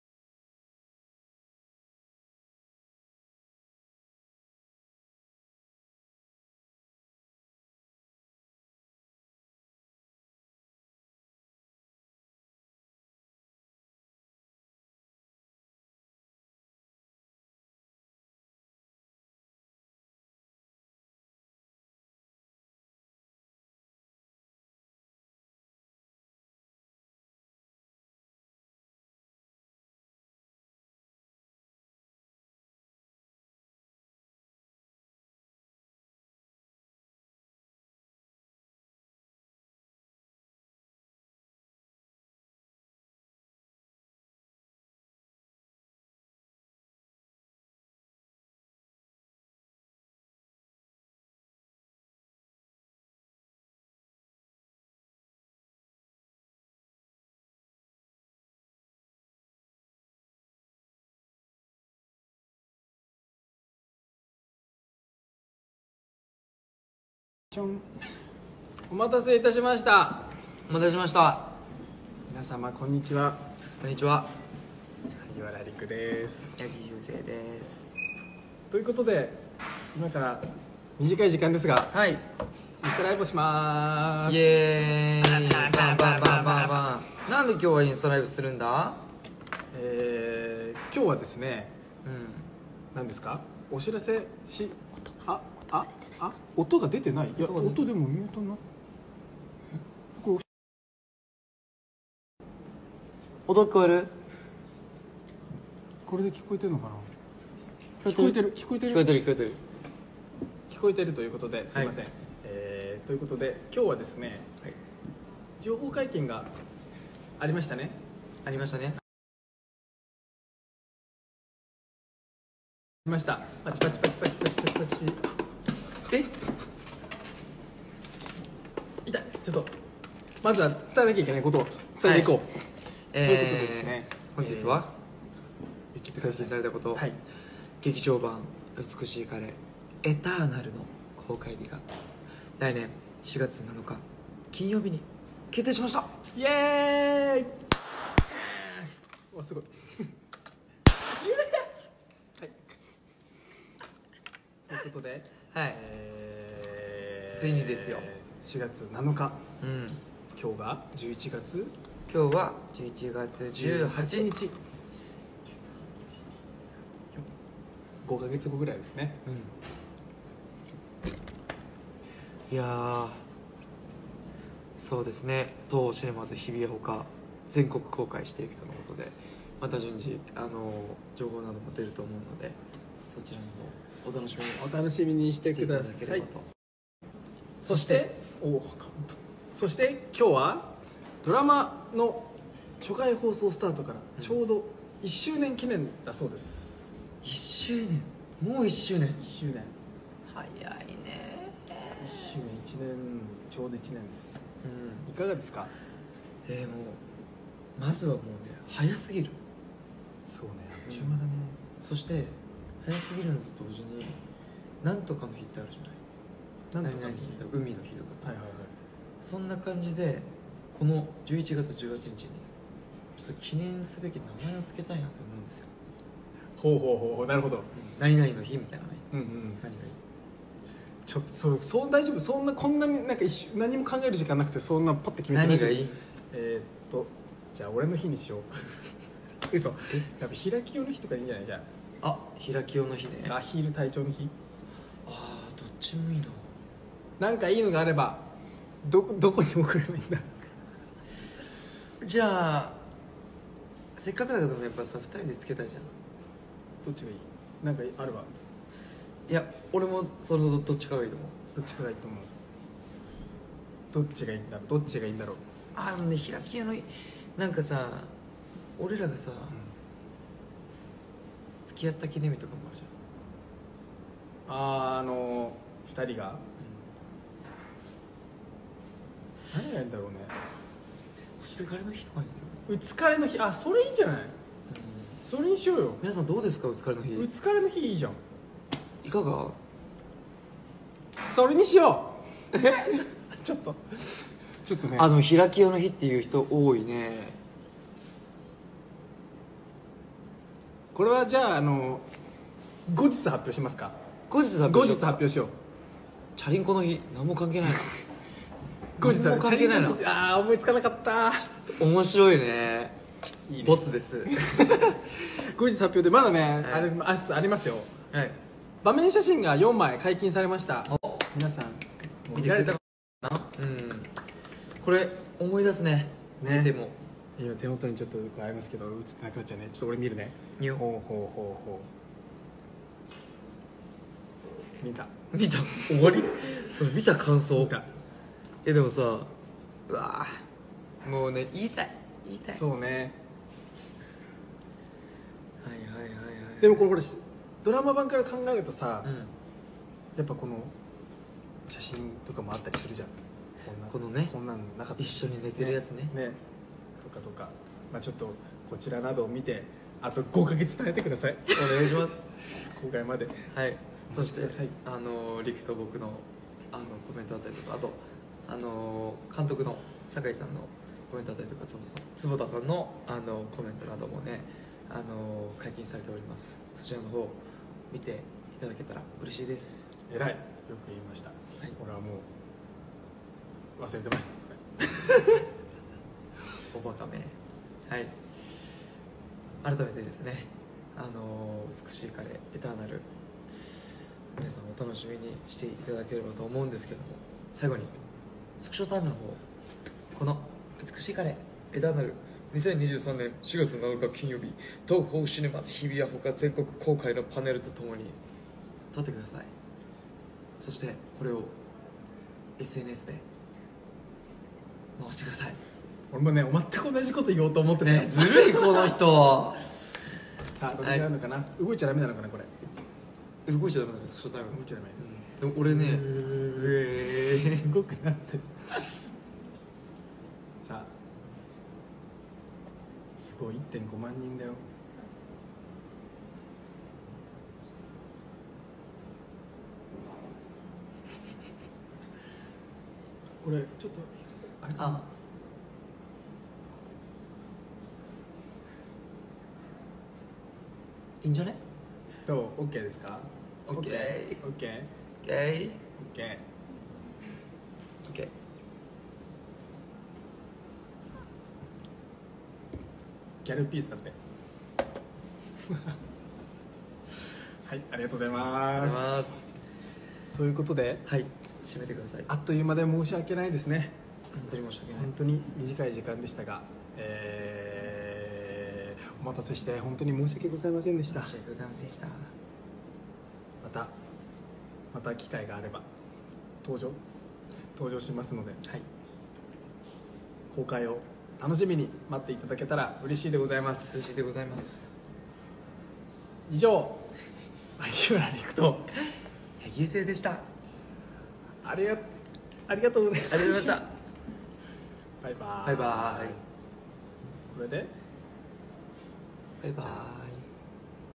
[ENG SUBS] 221118 Utsukushii Kare 1st Anniversary Live
rikuyuse celebrate utsukare's 1st year anniversary in a rushed instagram live while throwing in some flirting in between on yusei's part